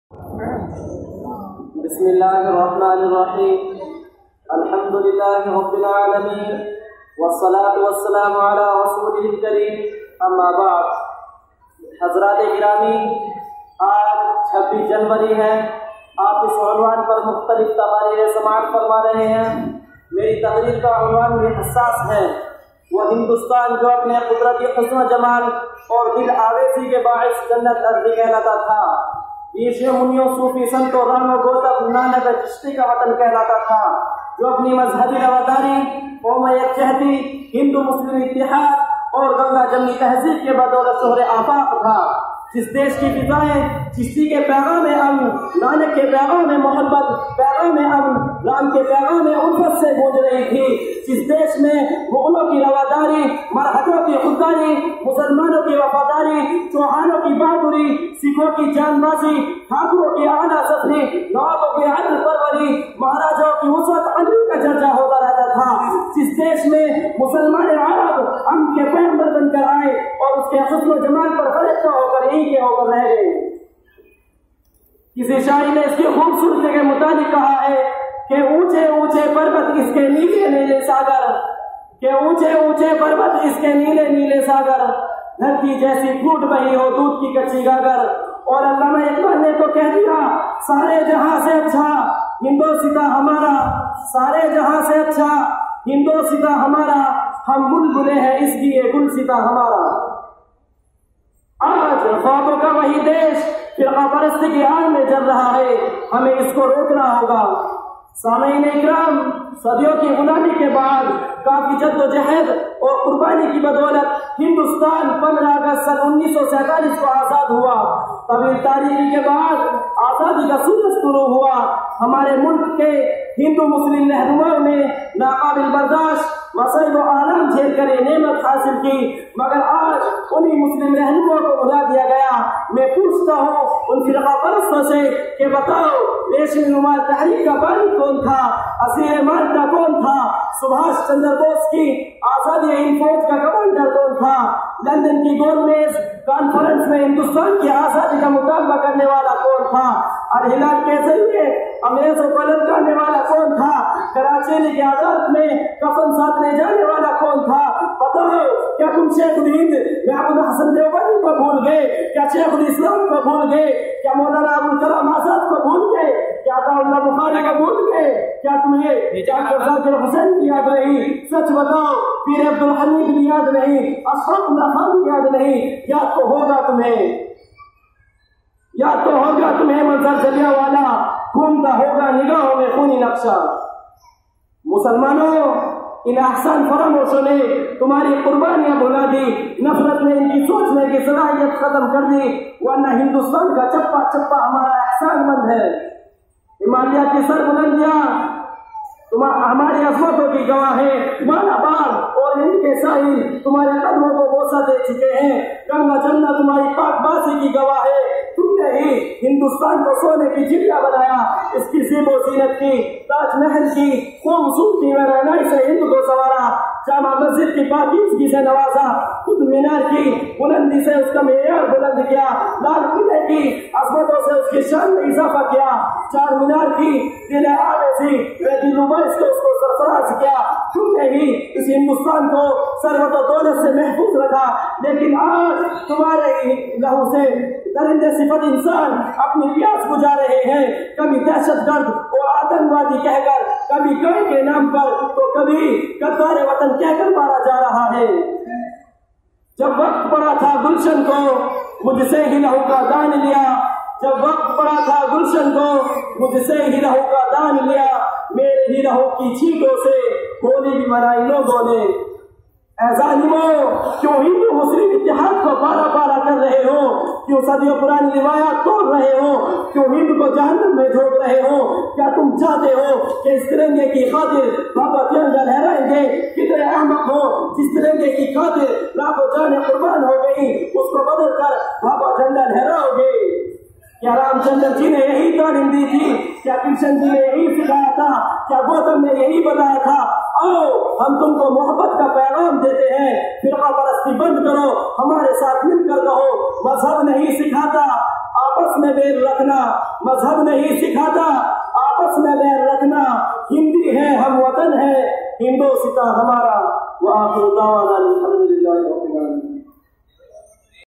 January 26 program
Giving a Student  speech on Republic Day